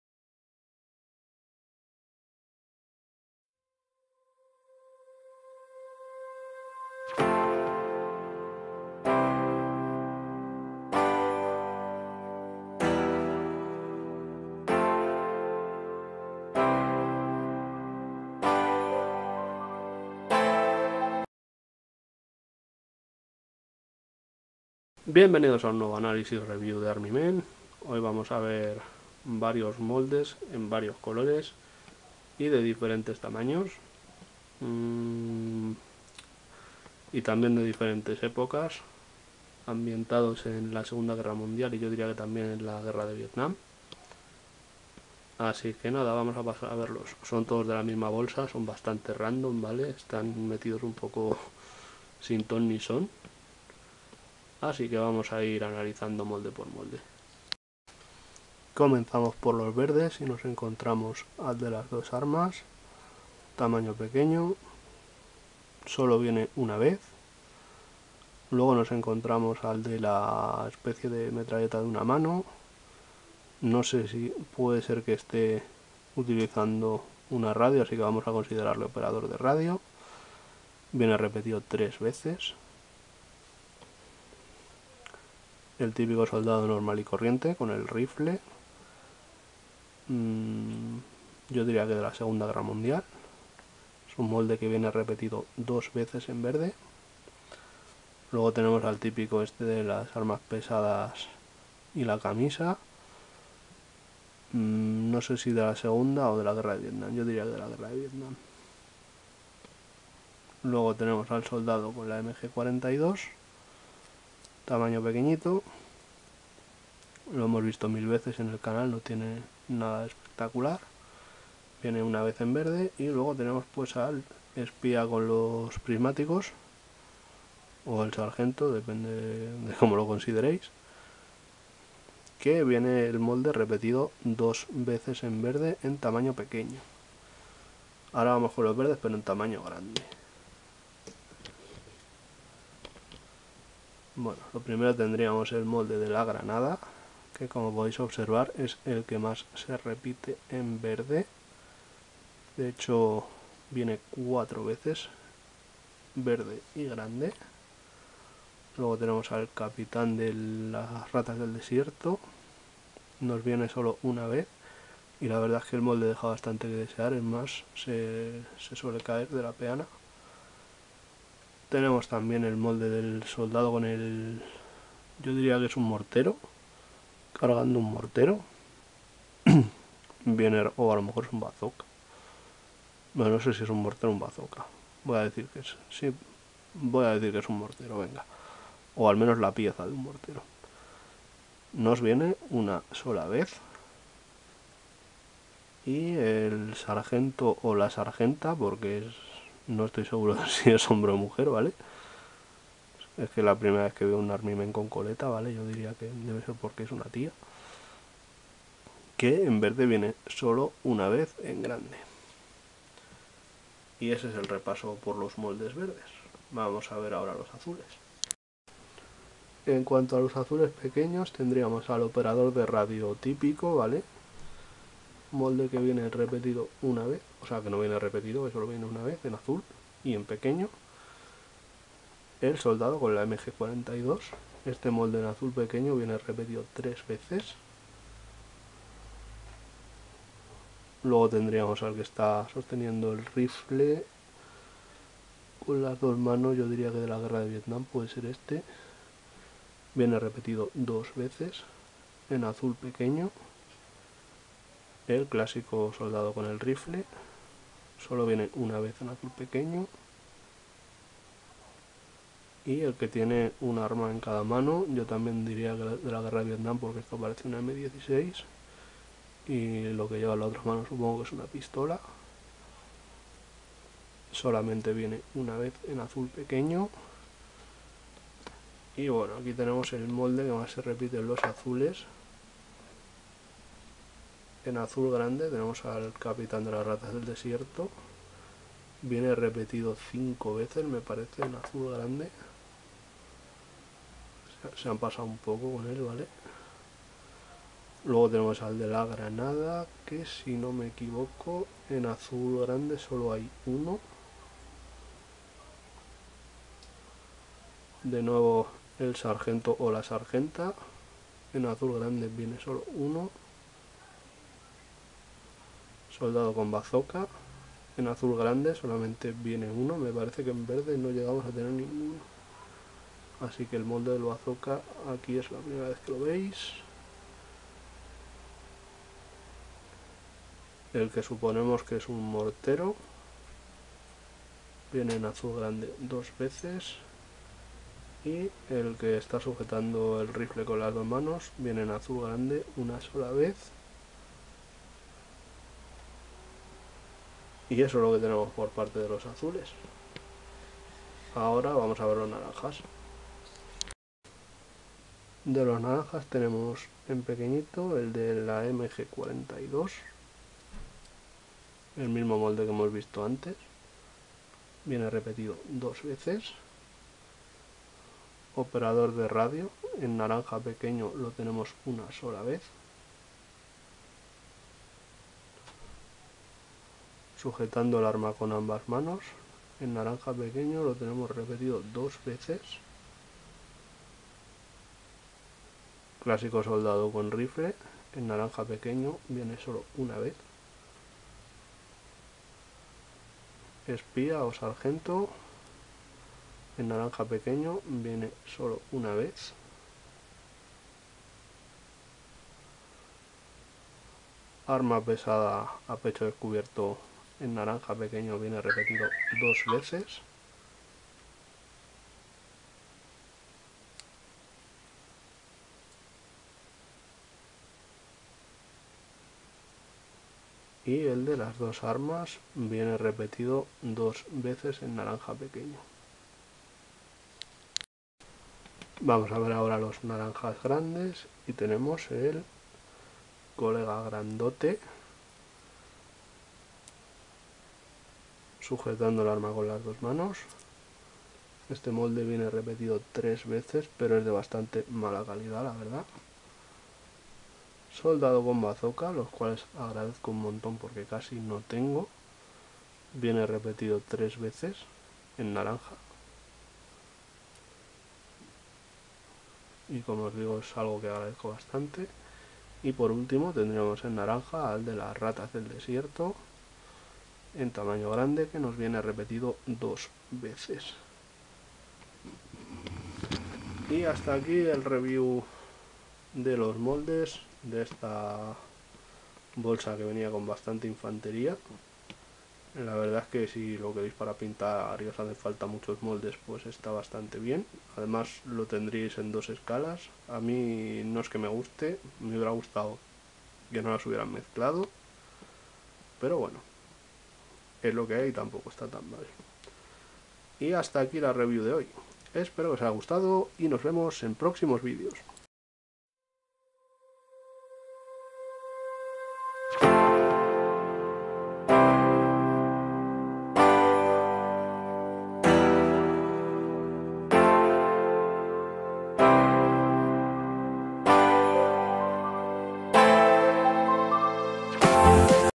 Pintando Facilmente con Josemi Review Army Men infanteria escala Mixta
Hoy analizaremos en detalle unos moldes color verde naranja y azul
de soldados de una bolsa que traia una escala grande y otra mas pequeña

¿Te gustan los Army Men? visita la web del canal
para mas Army Men;